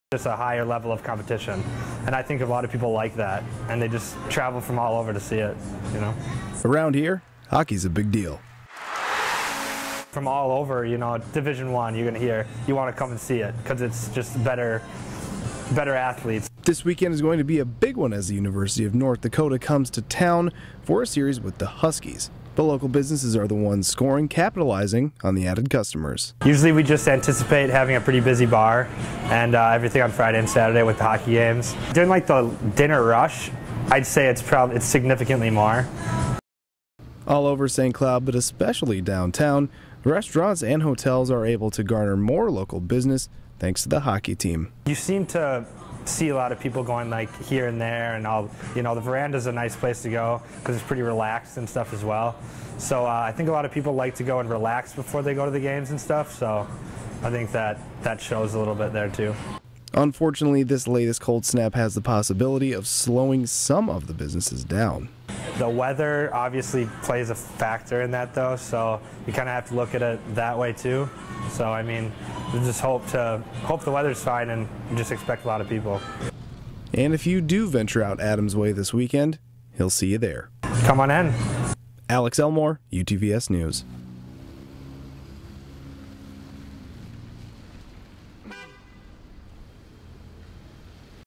Business Play
Alex Elmore caught up with one local business to see how they're cashing in on the popularity of the SCSU hockey team.